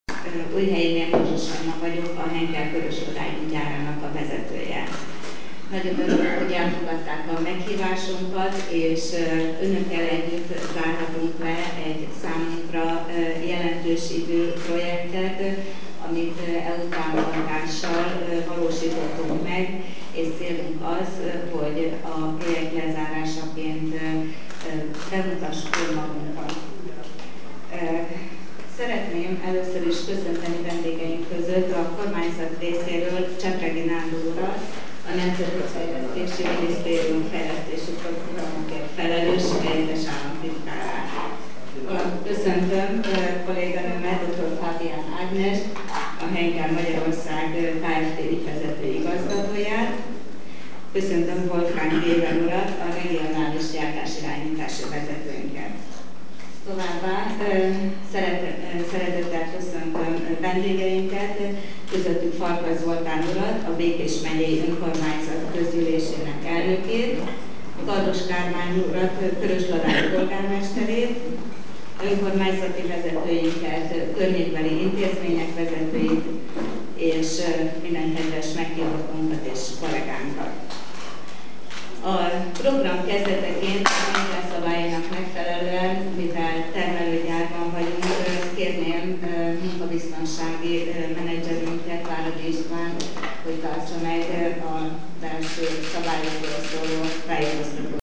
Henkel Magyarország - Ujhelyiné Mojzsis Anna 1
Henkel   Ujhelyiné Mojzsis Anna 1. Komplex technológiafejlesztési program fejeződött be a Henkel Magyarország Kft. Körösladányi Gyárában.
Az ünnepségen részt vett Wolfgang Weber a regionális gyártási igazgató, Csepreghy Nándor a Nemzeti Fejlesztési Minisztérium fejlesztési programokért felelős helyettes államtitkára, Fábián Ágnes a Henkel Magyarország Kft. ügyvezető igazgatója, Ujhelyiné Mojzsis Anna a Körösladányi Gyár igazgatója és meghívást kapott a sajtó.

Csepreghy Nándor a Nemzeti Fejlesztési Minisztérium fejlesztési programokért felelős helyettes államtitkára beszédében kitért arra, hogy a Henkel beruházása igazolja a kormány gazdaság fejlesztési elképzeléseit, új munkahelyeket hoz létre. Ez is mutatja a magyar gazdasági környezet stabilitását.  A helyettes államtitkár felhívta a figyelmet arra, hogy az EU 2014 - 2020 között a Magyarország számára lehívható, mintegy 8200 milliárd forint támogatás hatvan százalékát lehet majd közvetlenül gazdaságfejlesztésre fordítani. Az uniós forrásokra a Henkel beruházásához hasonló, innovatív, munkahelyteremtéssel együtt járó fejlesztésekkel lehet majd pályázni.

Wolfgang Weber regionális gyártási igazgató köszöntötte a résztvevőket, majd utalt arra, hogy az 1876-ban alapított Henkel vállalatnak mind a lakossági mind az ipari területeken vezető pozíciói vannak. Elmondta, hogy a bécsi székhelyű közép -- kelet -- európai és közép ázsiai regionális vállalat (Henkel Central Easten Europe) 32 országban koordinálja a vállalatcsoport tevékenységét. Megtudtuk, hogy a Henkel elsőbbségi részvényeit jegyzi a DAX német tőzsdeindex is. A vállalat 2012-ben közel 47 ezer munkatársával 16,510 milliárd euró árbevételt és 2,335 milliárd euró üzemi eredményt ért el. Majd kiemelte, hogy a Henkel innovatív, folyamatosan fejleszt és mivel lát a magyarországi gazdasági környezetben perspektívát, ezért a cég a beruházási költségek nyolcvan százalékát saját tőkéből finanszírozta.

Fábián Ágnes a Henkel Magyarország Kft. ügyvezető igazgatójától megtudtuk, hogy 2012-ben adták be az első pályázatot a termék gyártástechnológiájának kialakításához szükséges fejlesztőmunkára, a most lezárult második pályázat a gyártástechnológia megvalósítását és új munkahelyek teremtését segítette. Ehhez a 3,9 milliárdos projekthez 590 millió forintos támogatást nyert el a Henkel az Új Széchenyi Terv keretében. A pályázati anyagot a MAPI Magyar Fejlesztési Iroda Zrt. közreműködésével készítették. A beruházás összege tartalmazza többek között a gyártóüzem belső kialakítását, a berendezések megvásárlását, valamint a munkaerőre fordított költségeket.

Ezzel a beruházással 16-tal nőtt a foglalkoztatottak száma is -- mondta Ujhelyiné Mojzsis Anna a gyár igazgatója.
Megtudtuk azt is, hogy a Körösladányi Gyár 1993-ban a Henkel Magyarország Kft. önálló egysége lett. Az itt gyártott ismert márkatermékek a Bref toalett tisztítók, a Somat folyékony gépi mosogatószer, a Sofix padlóápolók és Opti bútorápolók, és az új Persil Duo -- Caps mosószer. Ennek az újonnan kifejlesztett magas minőségű terméknek az innovációja abban rejlik, hogy a gél állapotú összetevők két külön kamrában vannak fóliázva és csak vízben oldódnak fel. A Henkel csoporton belül csak Magyarországon -- Körösladányban - gyártják és innen exportálják több mint húsz európai országba.